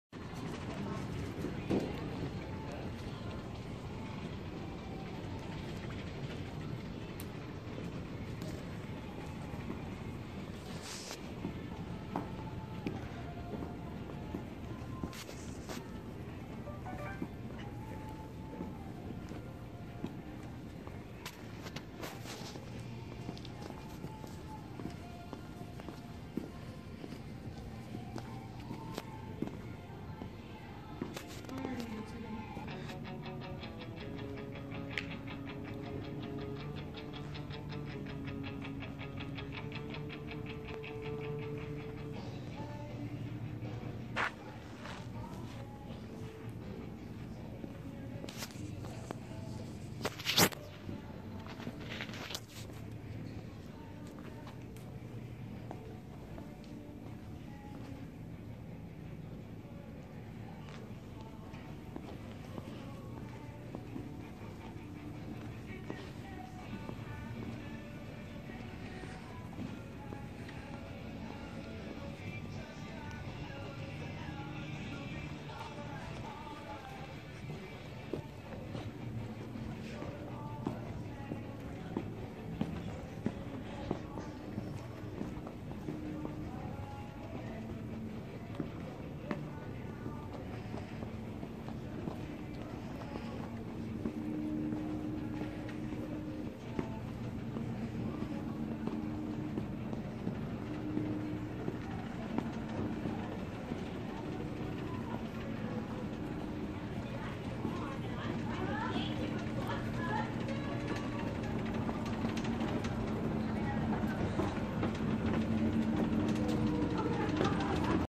Macy's Terre Haute Last Day
3-8-18 was Macy's Terre Haute's last day. The upper floor stuff was moved to the bottom floor. The upper floor is selling all the clothes racks, signs, mirrors and tables. Interesting to see this place empty. All video is in the upstairs.